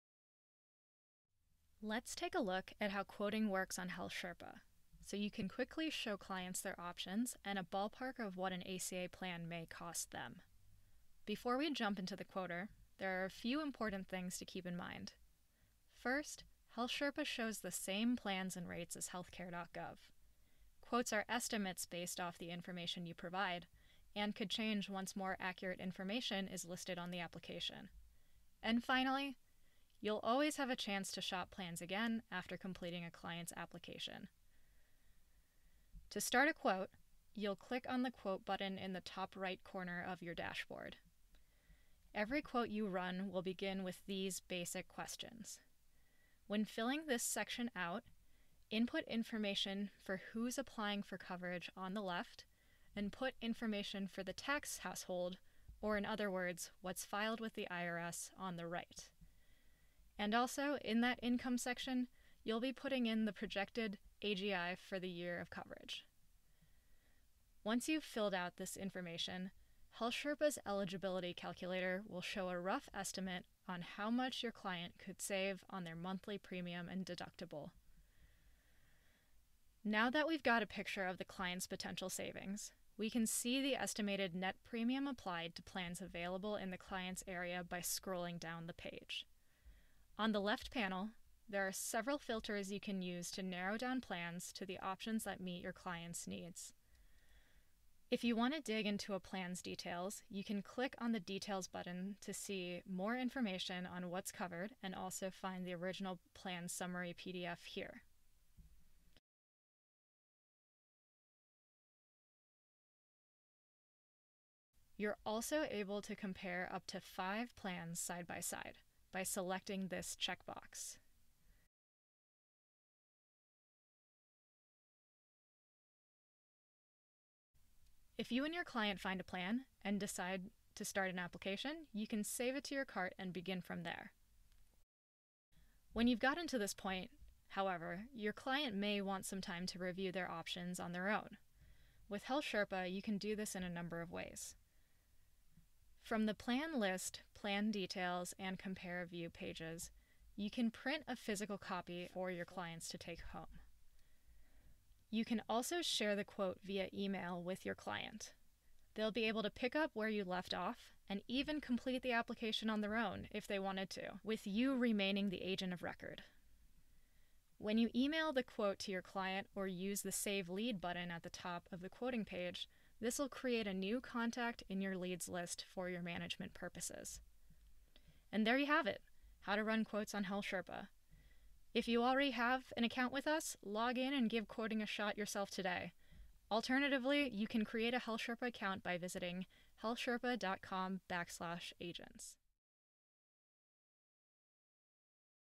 How to run an ACA quote with a HealthSherpa for Agents account
In this video, we cover how ACA agents can quickly show clients their options and a ballpark of what an ACA plan may cost them.

Video Chapters:
0:00 Intro to quoting
0:34 Starting a quote
1:17 Filter and compare plan options
2:24 Share and save quotes
2:57 Try it out yourself!